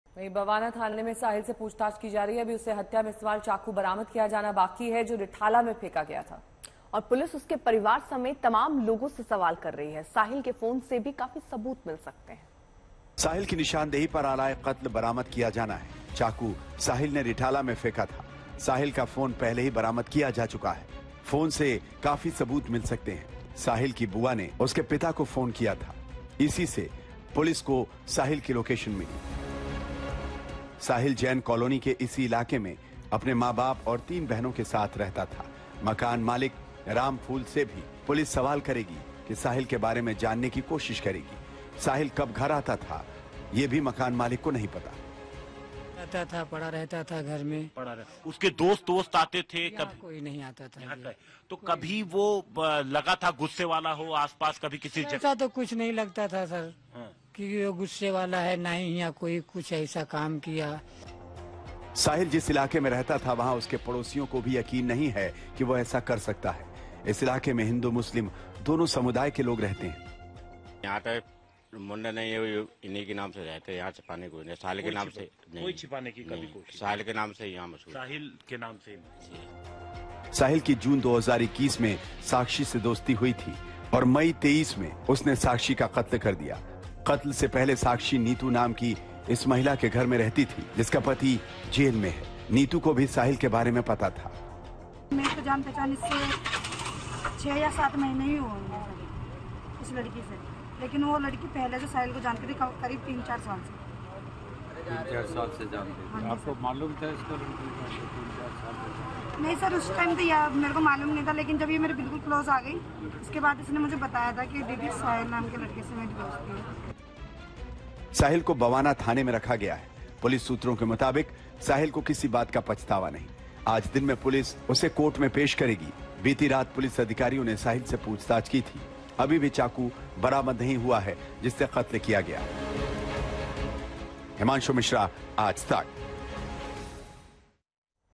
Delhi Sakshi Murder Case: 'साक्षी-साहिल में चल रही थी लड़ाई', मृतका की दोस्त नीतू ने क‍िए खुलासे
राजधानी दिल्ली में नाबालिग लड़की साक्षी की हत्या कर दी गई है. इस वारदात को अंजाम देने वाले लड़के की पहचान साहिल के रूप में हुई है. साक्षी की हत्या पर उसकी दोस्त नीतू ने बयान दिया है. देखें ये वीडियो.